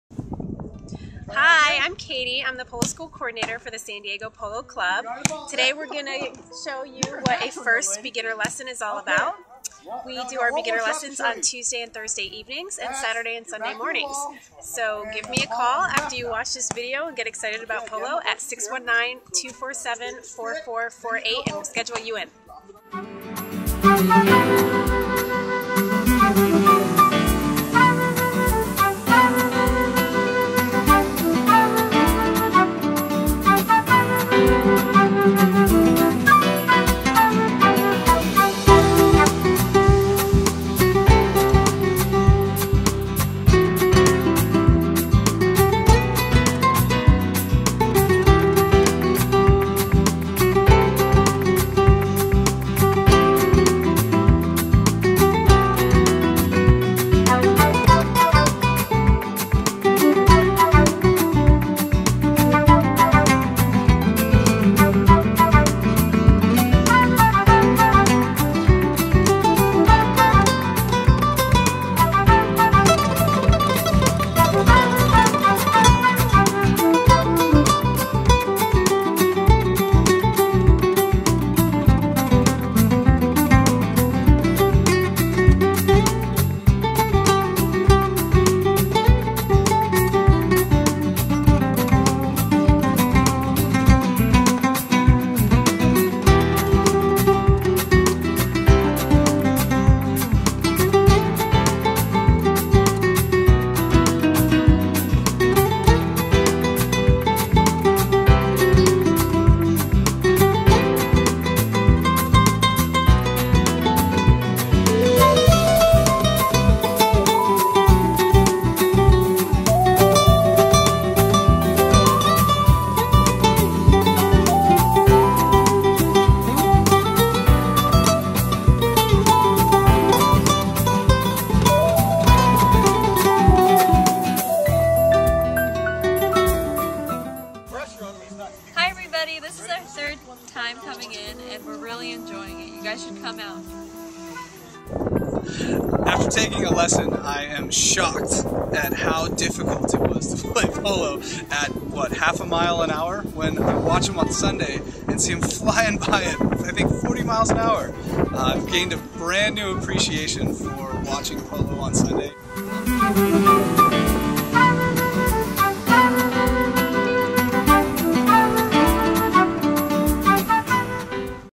SDPC First Lesson
Check out what your first polo lesson is all about.  Start out by learning how to hit the ball on foot with a short mallet.  Then, mount one of our safe school horses and head into the arena for a little practice.  At the end of hour lesson, play a short scrimmage against your teammates.
Wear closed toe shoes or boots and pants.  If you have a helmet, bring it, otherwise, we'll supply it.
Your first lesson is free for southern CA residents.  Valid ID required.
Ages 18+.